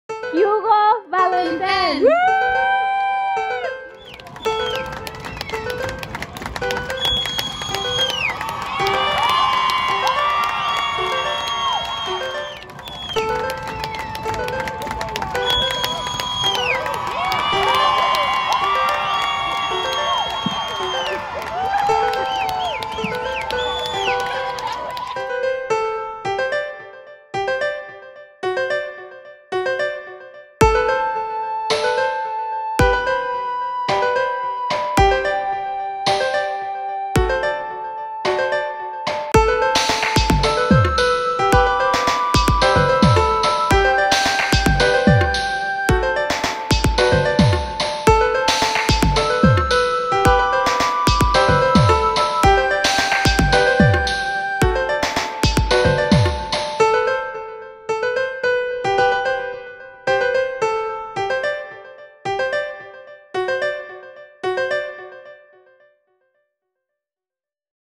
Hugo Valentin Jr: Caps Off To A Purple Graduation 🎓
Welcome to my graduation video, I am so proud to have finally finished high school and I’m excited to see where life takes me from this point, I will most definitely pursue my dreams of acting, singing, cinematography, choreography, and more. I hope you enjoy!